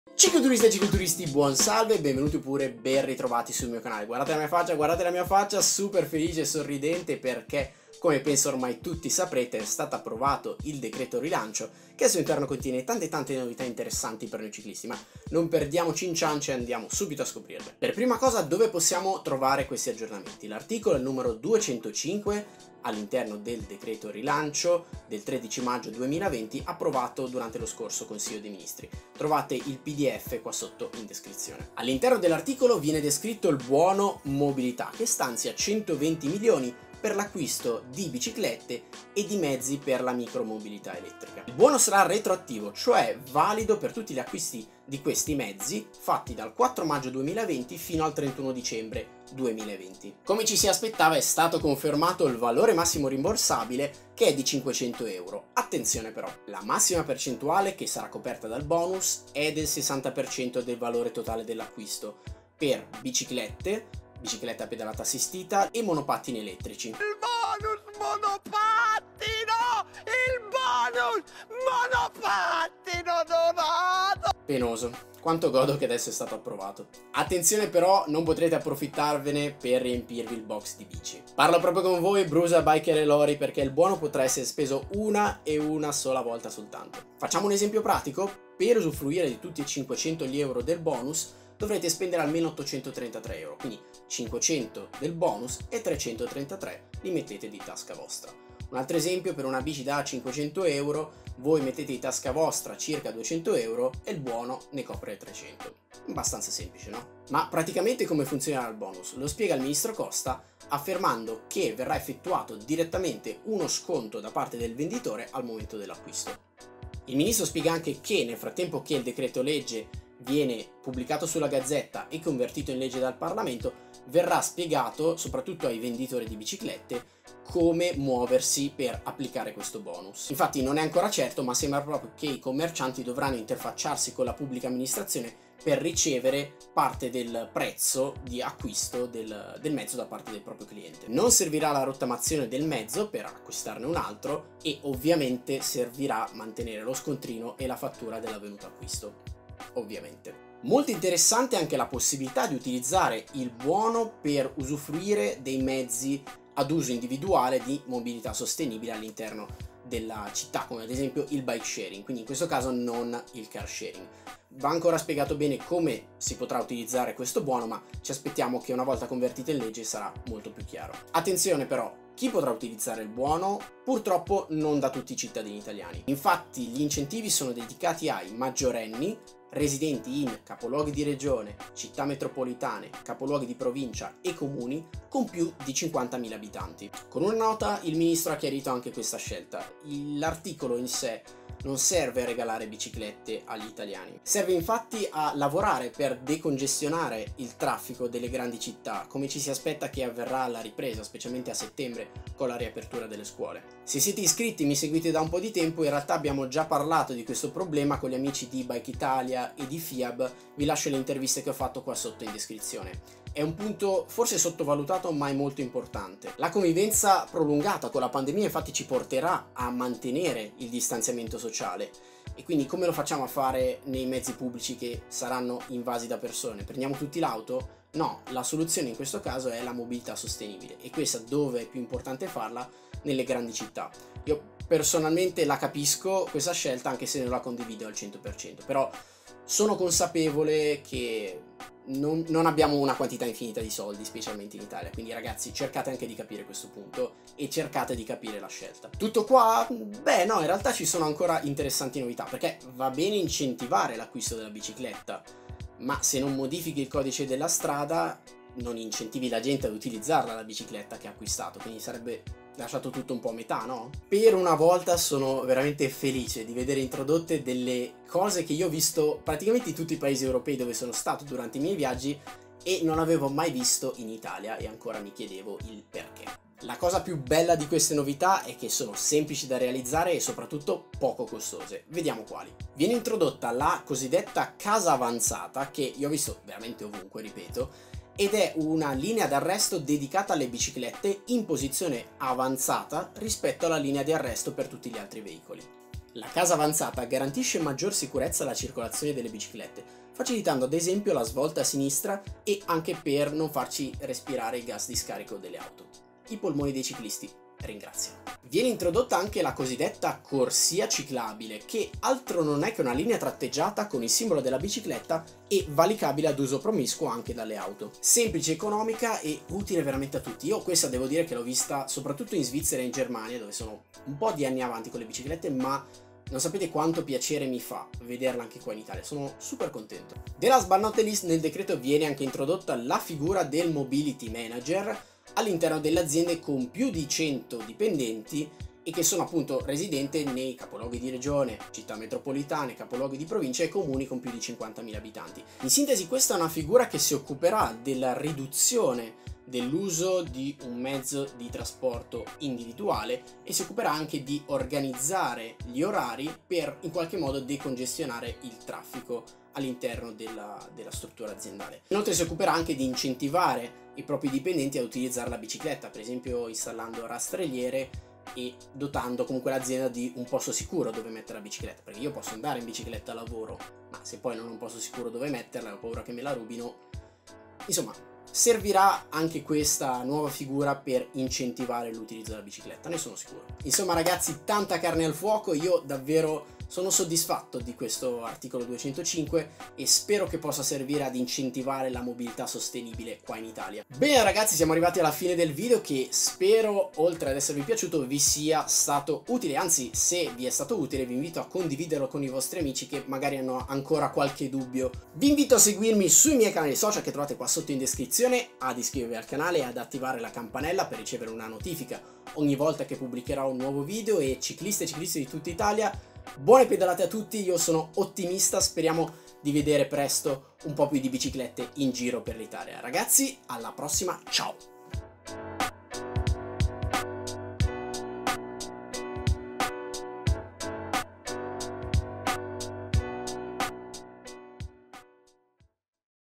500€ PER ACQUISTARE BICICLETTE? IL BONUS MOBILITÀ SPIEGATO SEMPLICEMENTE
Grosse novità per noi ciclisti e amanti della bicicletta! Nel "Decreto Rilancio" approvato il 13 Maggio 2020 sono spiegati gli incentivi all'acquisto di mezzi per la mobilità sostenibile come biciclette, biciclette a pedalata assistita e monopattini elettrici.
Si parla di 500€ per acquistare una bicicletta, ma... è proprio così? E come sono vengono erogati gli incentivi? Chi può accedere agli incentivi per acquistare una nuova bicicletta o un monopattino elettrico?

In questo video facciamo chiarezza su come sono strutturati non solo gli incentivi ma anche i cambiamenti al codice della strada...

Si parla tanto del bonus di 500€ per noi ciclisti!
Amazon spiega qui come accedere al bonus mobilità

🛴 Monopattini elettrici
🚵 Bici Elettriche

Filmato con LUMIX LX15
Schede di memoria: SD da 64GB
Montaggio con Adobe Premiere Pro